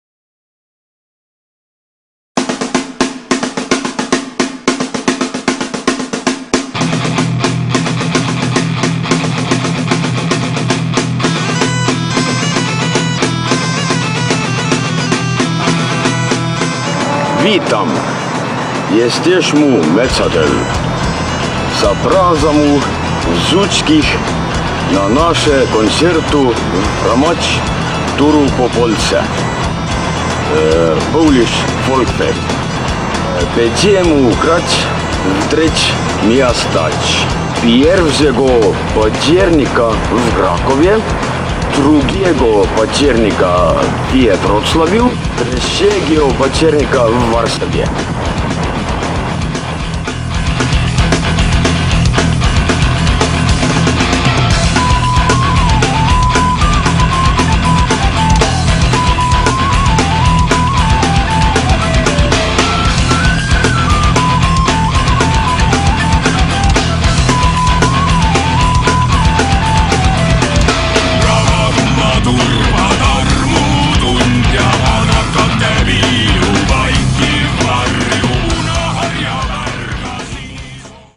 Metsatöll zaprasza na polski "Folk Fest" 2015
Folk Fest 2015 (Ensiferum, Skyforger, Metsatoll + supports)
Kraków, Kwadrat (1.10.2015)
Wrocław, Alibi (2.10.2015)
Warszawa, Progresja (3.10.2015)